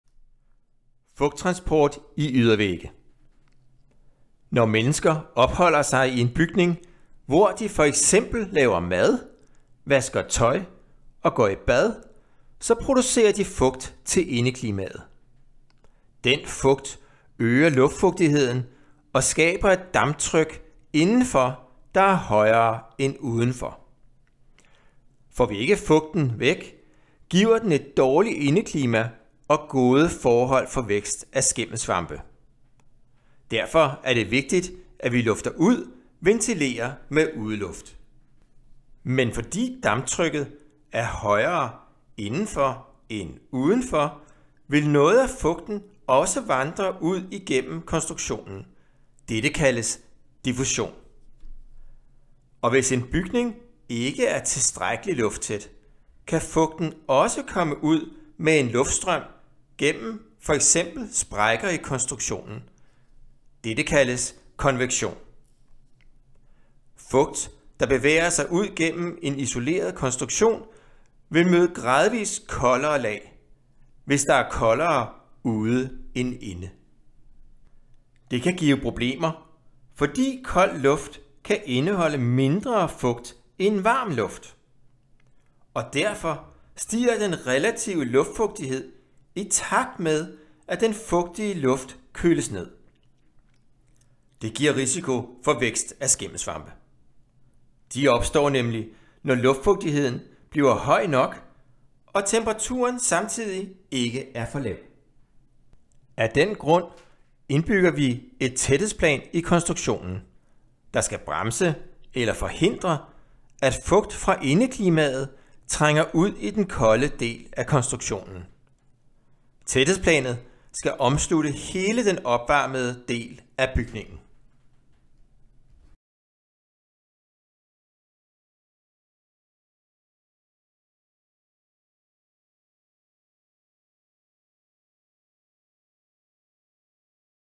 Fugttransport i ydervægge
Her kan du se en video hvor Torben Valdbjørn Rasmussen, seniorforsker ved BUILD, fortæller om fugttransport i lette Ydervægge. Videoen er produceret til projektet, Vidensbaseret Grøn Omstilling af Tømreruddannelsen (VIGOT).